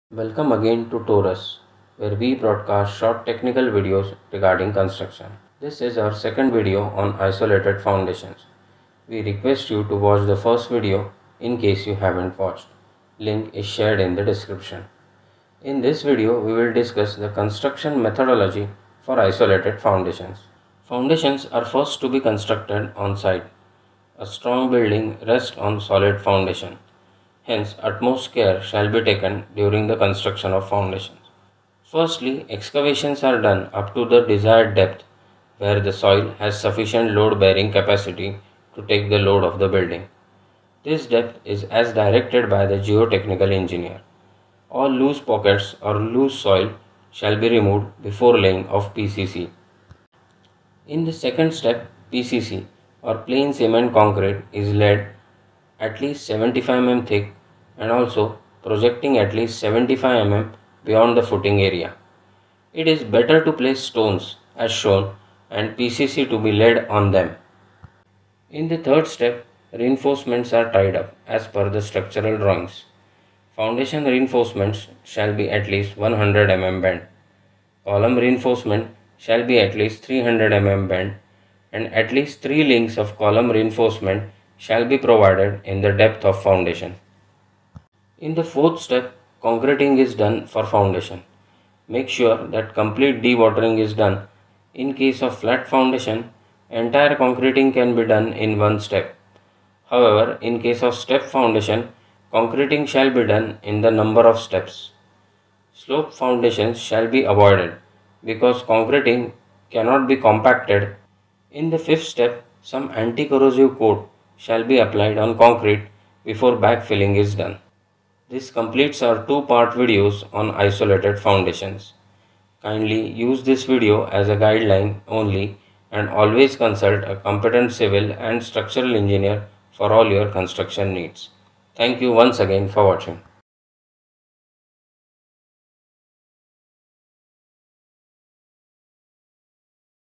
Isolated Foundations (Construction Methodology)
The video explains the basic construction methodology for Isolated foundations. The entire process of Casting of Foundations is explained through the use of graphics. This video is primarily intended for students of civil engineering. The Link for Video on Design Philosophy is shared here.